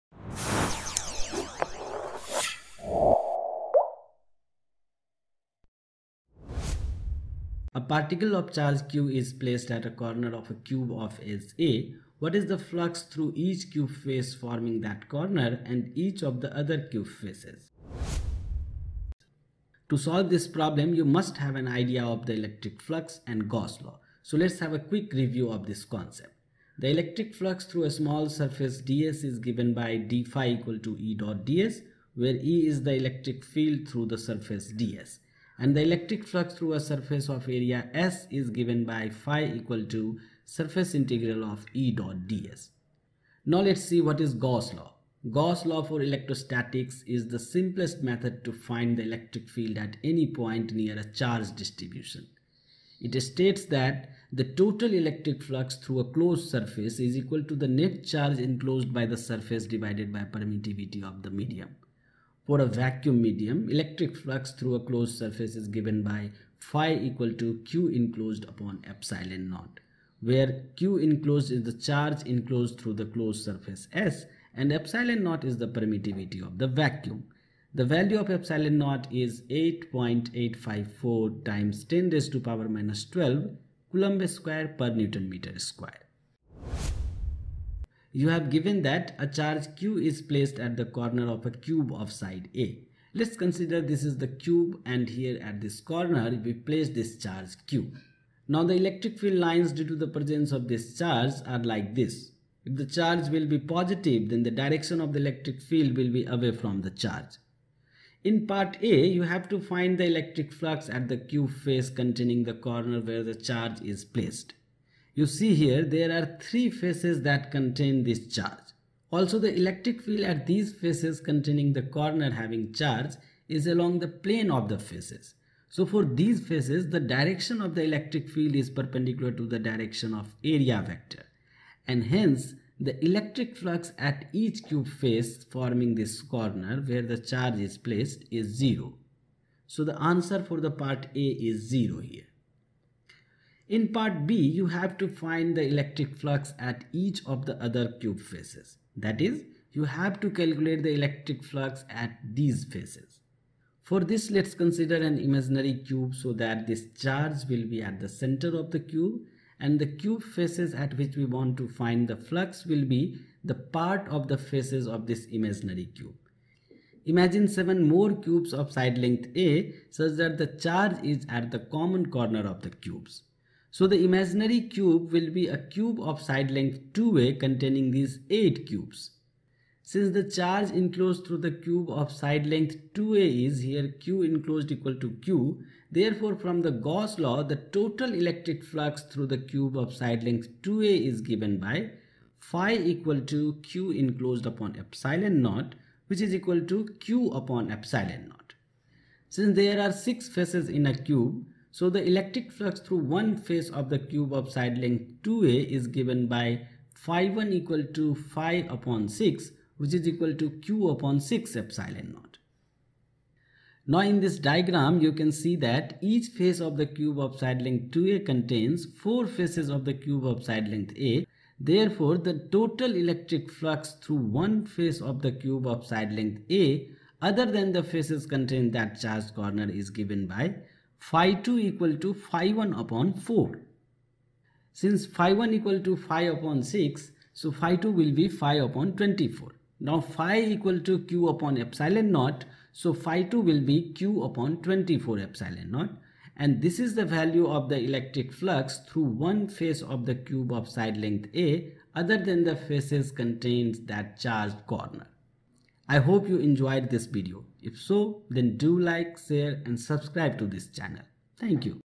What is the flux through (a) each cube face forming that corner & (b) each of the other cube faces?
A particle of charge q is placed at a corner of a cube of edge a. What is the flux through (a) each cube face forming that corner and (b) each of the other cube faces?